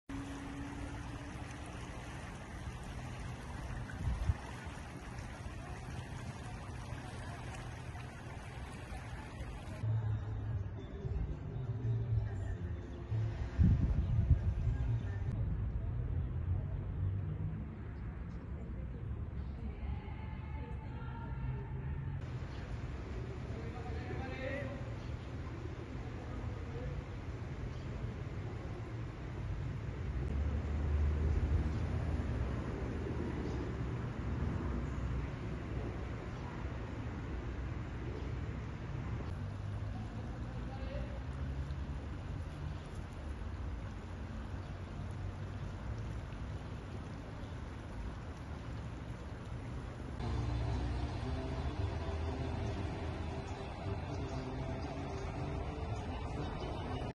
Presenta altos niveles de contaminación el río ZahuapanVía:Richard Castillo//EsImagen//
Presenta altos niveles de contaminación el río Zahuapan
Vía:Richard Castillo//EsImagen//